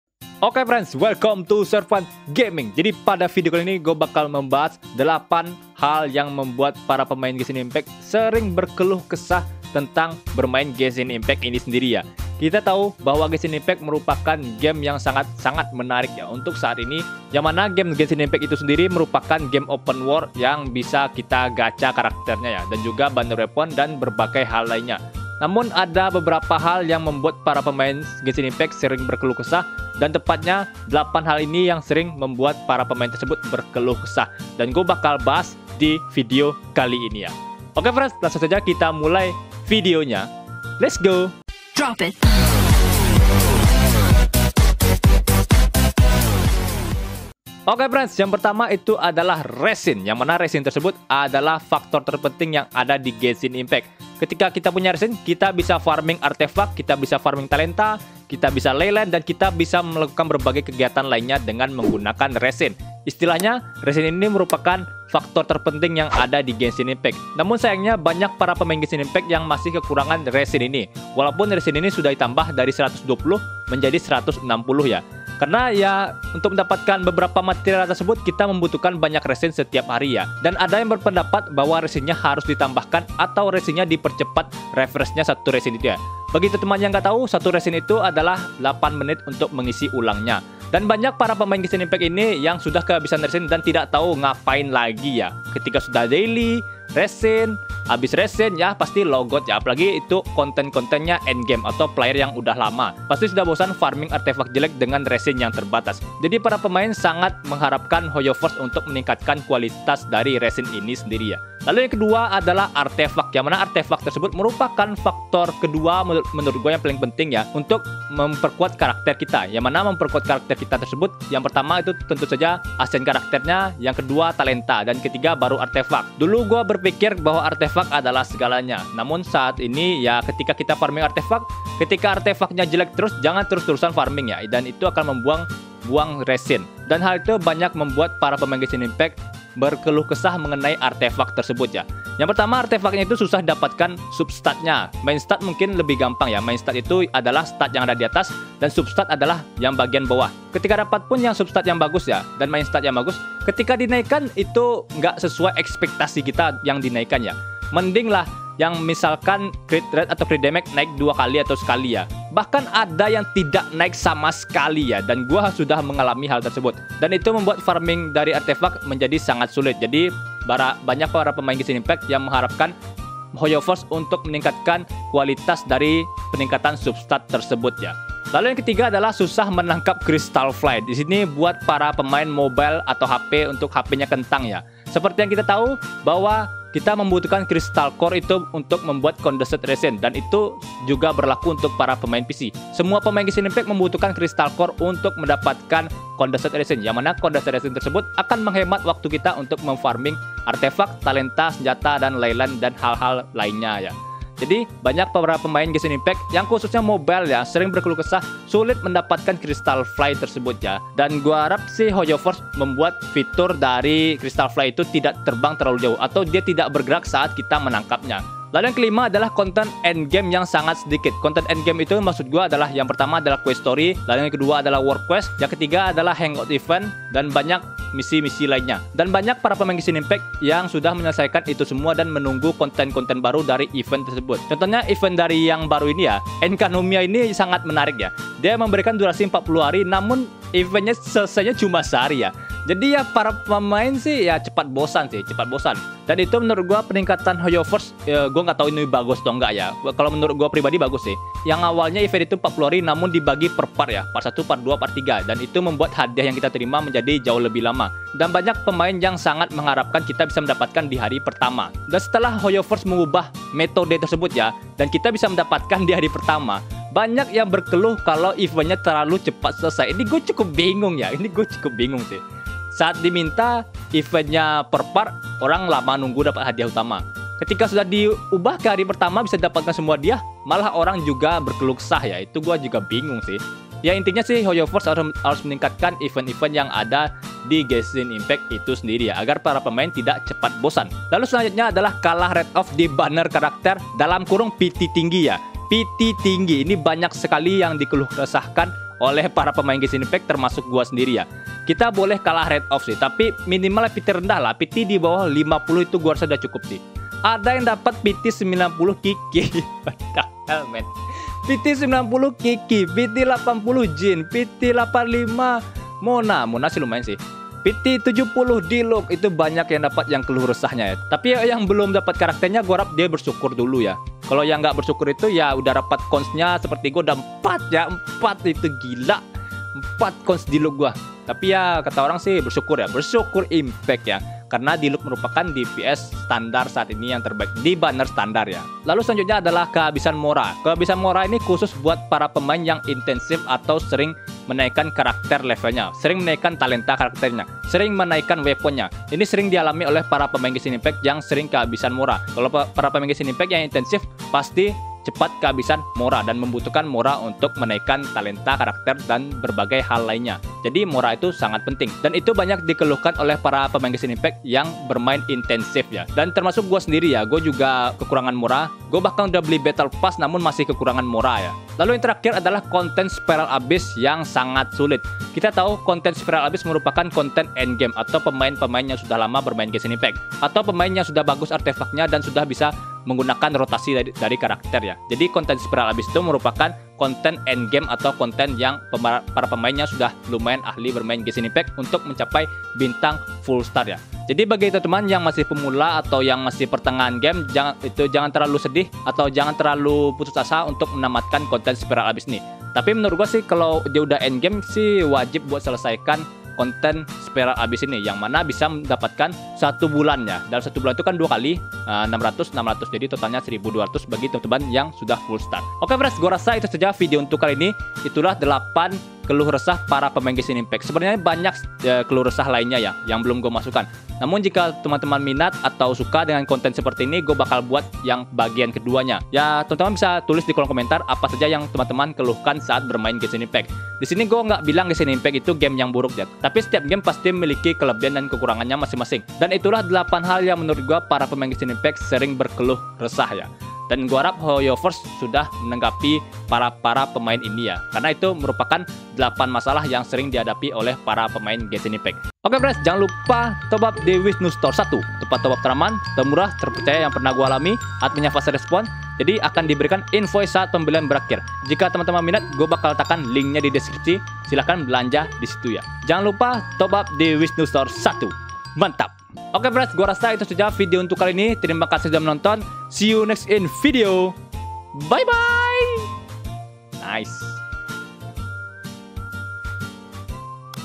CEPAT BOSAN? 8 KELUH RESAH PARA PEMAIN GENSHIN IMPACT
OK Friends! Pada video kali ini gua mau membahas keluh resah yang banyak keluhkan para pemain genshin impact, baik itu masih pemula maupun yang sudah endgame di CEPAT BOSAN? 8 KELUH RESAH PARA PEMAIN GENSHIN IMPACT. Enjoy Watch!